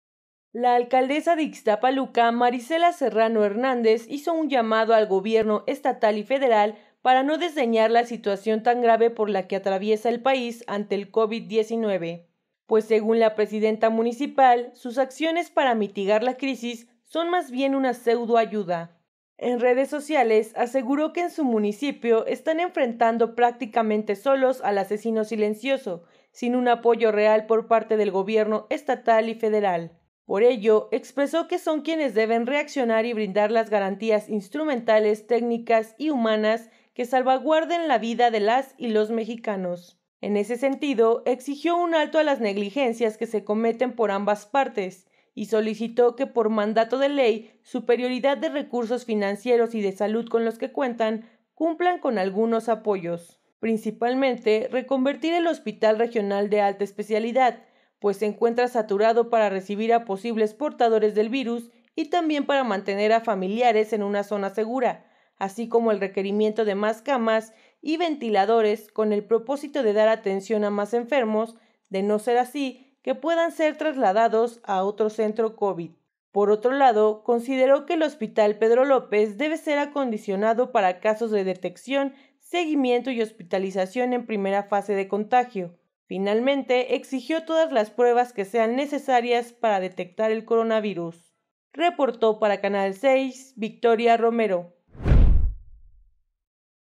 ▶️ Del Mazo y AMLO están simulando apoyo médico, denuncia alcaldesa de Ixtapaluca
▶️ Edomex | Del Mazo y AMLO están simulando apoyo médico, denuncia alcaldesa de Ixtapaluca